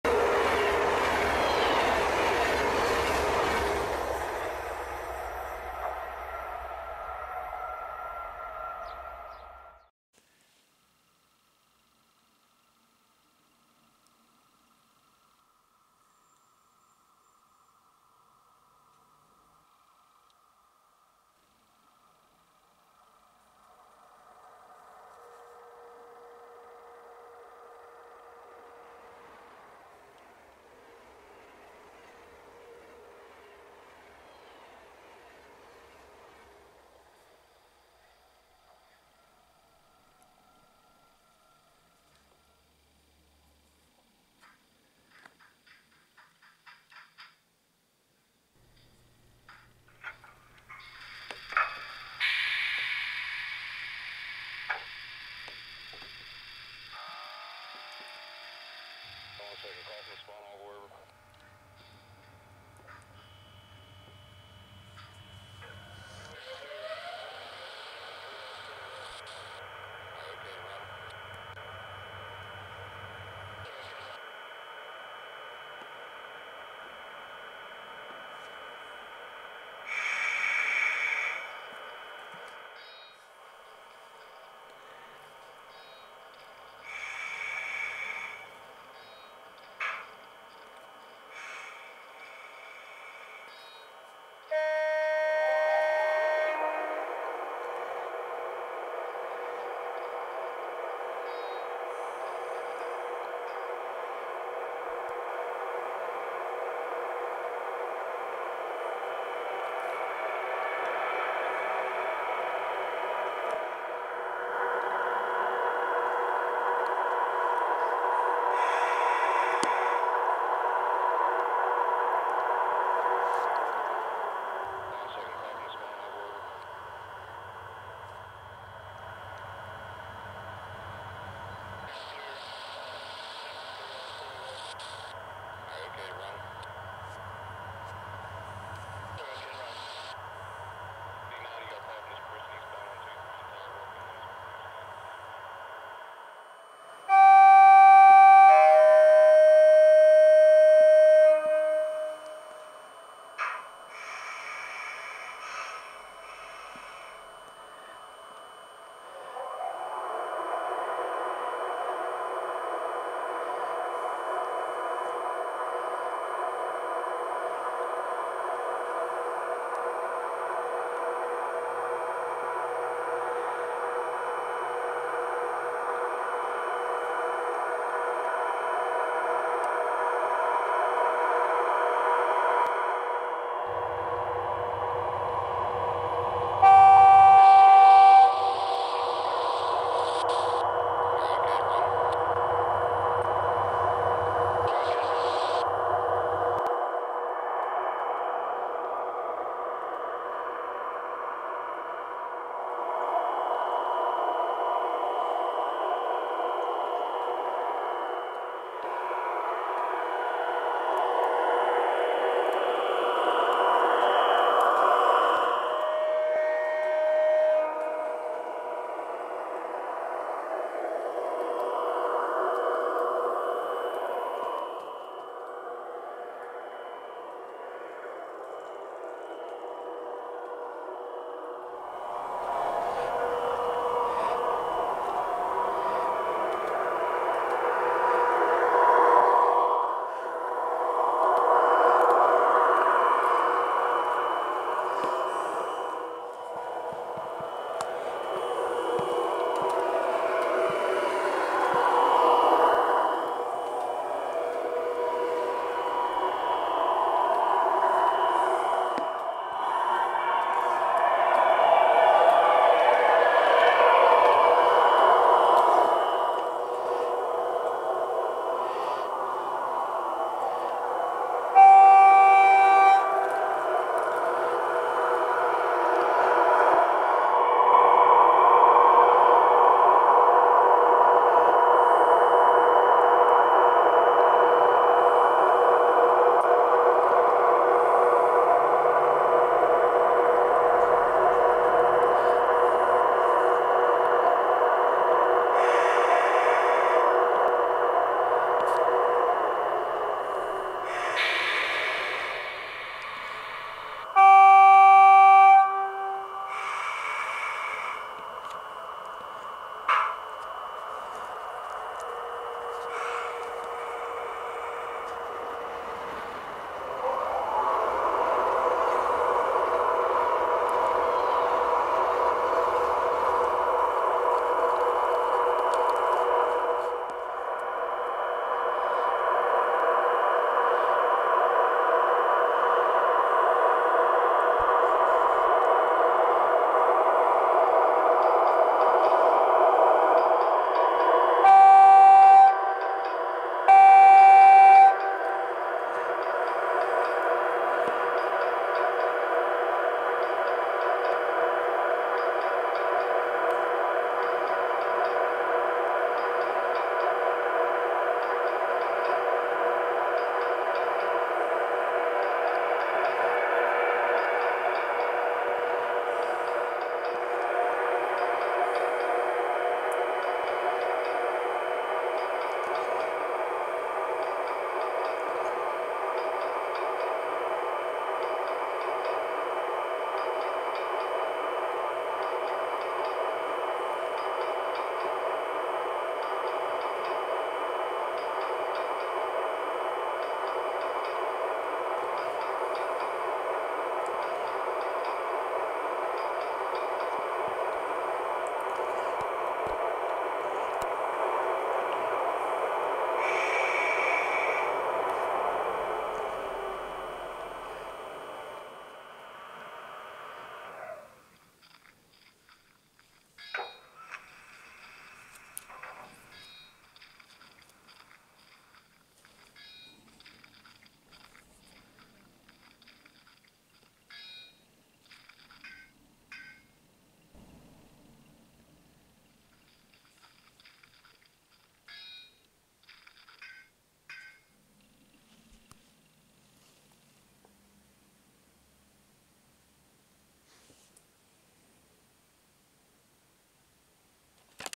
The Experiment...!
This video is a simulation of the testing of a new Kinetic power system for
all main line locomotives! Rington yard has pulled a pendolino power car
out of mothballs for the test, it is also fitted with an auto track monitoring
system with adjustable speed setting for remote backing up on to sidings
by the driver. as usual its attracted a lot of intrest...enjoy !